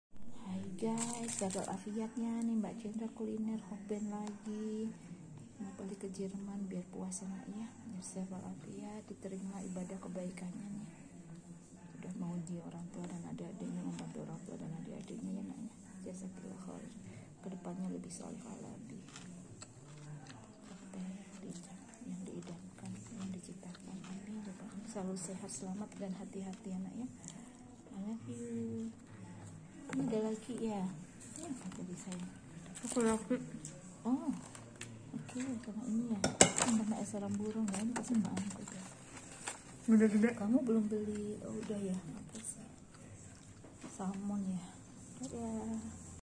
HOKBEN
MBAK CENDRA BELIKAN AKU iPHONE AGAR KOMUNIKASI DAN BEKERJA LEBIH LANCAR AAMIIN YRA

AKSESORI DIBELIKAN MAS SAID

JUGA NEXT LAPTOP BARU DAN TAS YANG H ITUHHH

JAZAAKUMULLOOH KHOIR

FARWELL

C U AGAIN

PASTI SEDIH

TAPI INILAH BERJUANG!!

MBAK CENDRA INSYAA ALLOOH PULANG LAGI FEBRUARI 2023 PAS AKU ULTAH 54 LIHAT KONSER WESTLIFE DI JAKARTA

DIA PUN LIHAT KONSER WESTLIFE DI IRELAND

DI JAKARTA KALI INI TAMBAH BAHAGIA LIHAT KONSER TULUS DLL

DAN KETEMUAN LAGI INSYAA ALLOOH JAMBORE PRAMUKA SEDUNIA DI KOREA AGUSTUS 2023

NGULINER TERUS SEBELUM MBAK CENDRA BALIK KE JERMAN🇮🇩🇩🇪💚⭐🌹SEMOGA SEMAKIN SHOLIHAH DAN DIMUDAHKAN MENGGAPAI YANG DICITAKAN🪄🌷AAMIIN YRA💐

Happy selaluh🌺sehat wal afiat ya gaes🌸aamiin🙂